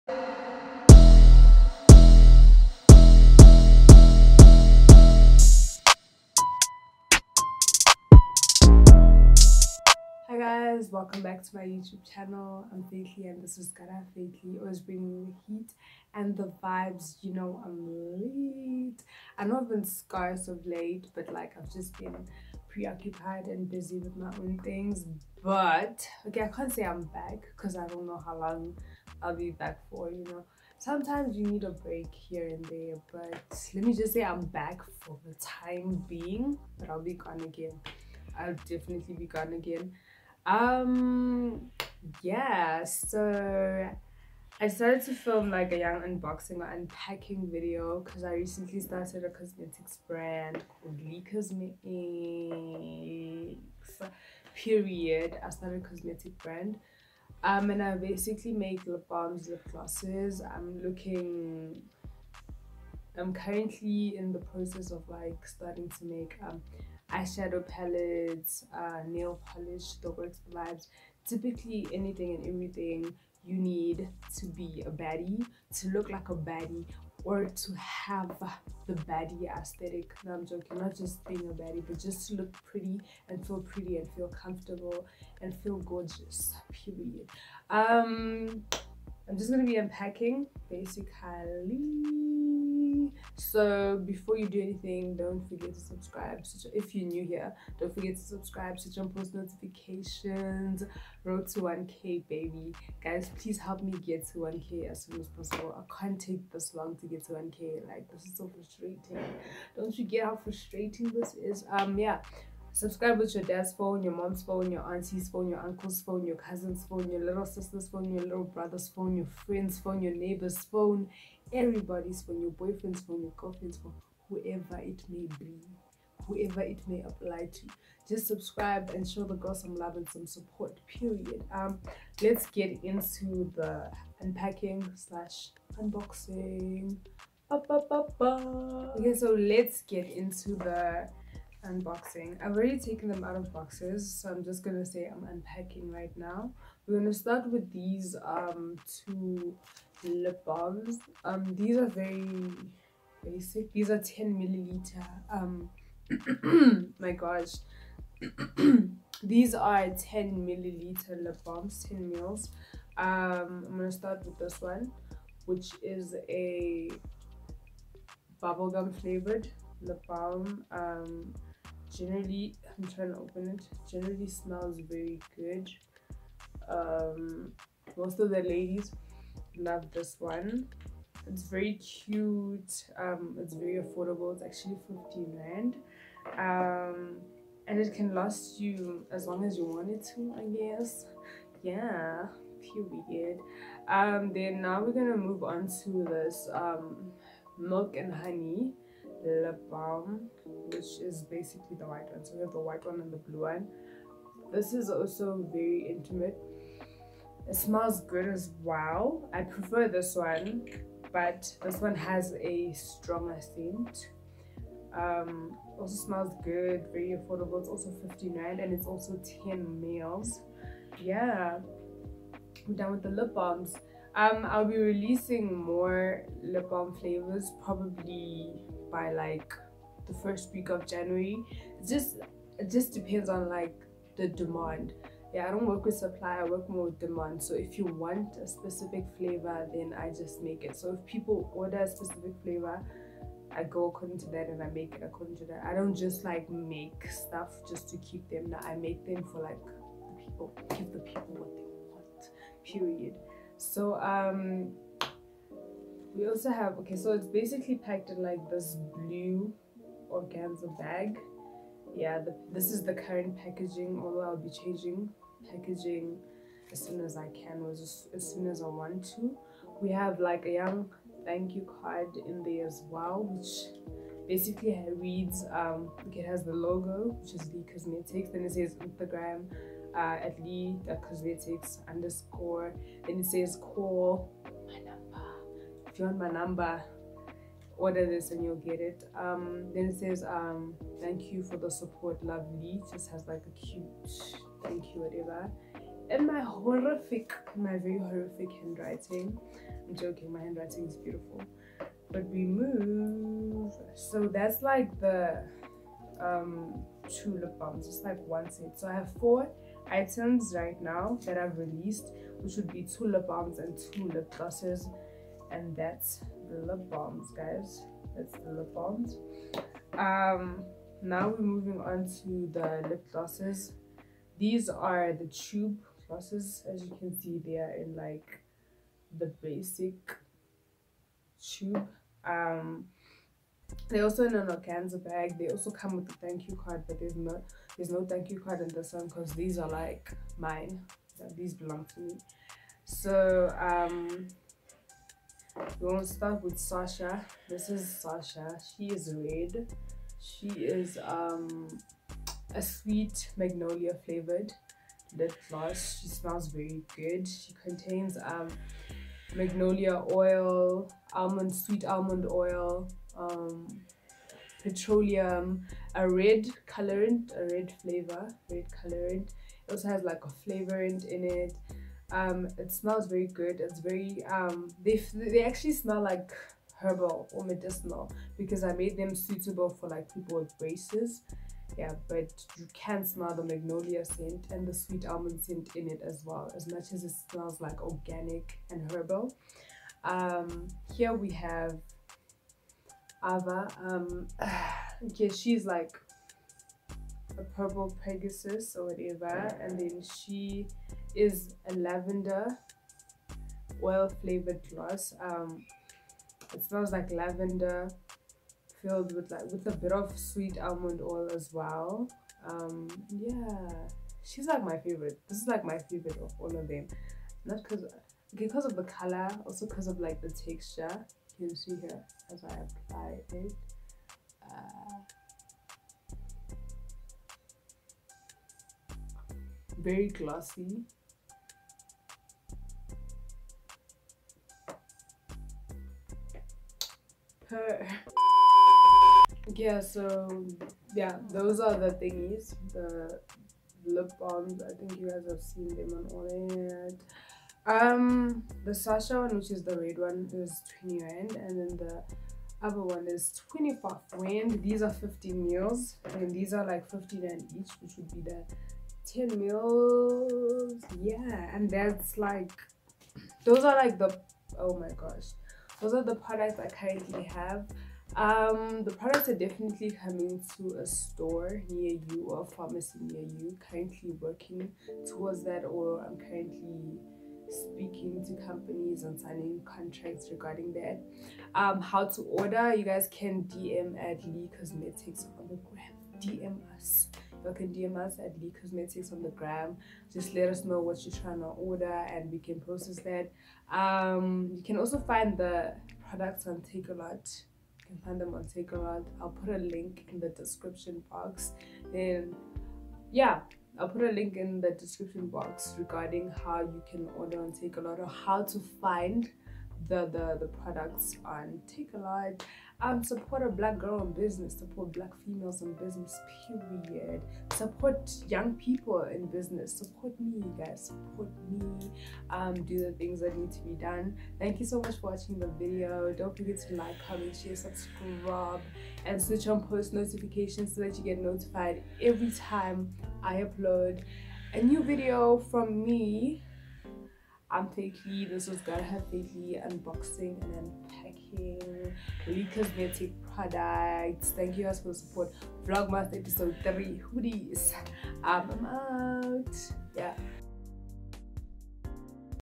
Starting a Cosmetic Brand in South Africa ✨ PART 1
thank you for the support ❤ I love you

hi yall 💚 so I recently started a Cosmetic brand called Lee Cosmetics & in this video I showcase a few of the products I currently have in stock.

Pricelist:
Bubblegum lip balm 10ml : R15
Milk & honey lip balm 10ml : R15
Sasha Lip Gloss 15ml : R20
Ava Lip Gloss 15ml : R25
Delivery : R60

Please note all products are currently available only in South Africa.

Non copyright to background music
Personal use permitted .